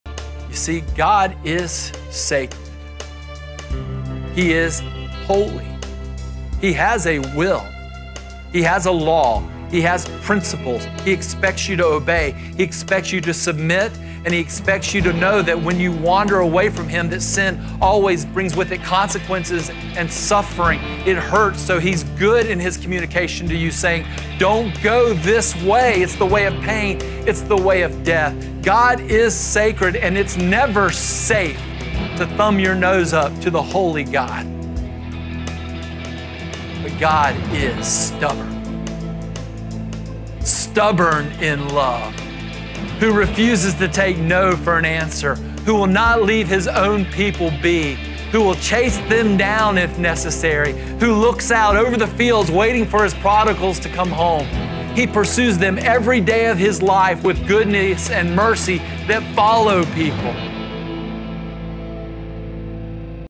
Sacred, Stubborn, & Sovereign / Joe Franks
While Saul seeks help from a witch, David likewise becomes anxious and seeks refuge with the enemies of God. He forgets God's faithfulness and wanders away to live with the king of Gath. His sin catches up to him, and he suffers the consequences. But God, who is sacred, is also stubborn in love. He pursues David by His grace. In His sovereignty, He gifts David with repentance and renews his faith.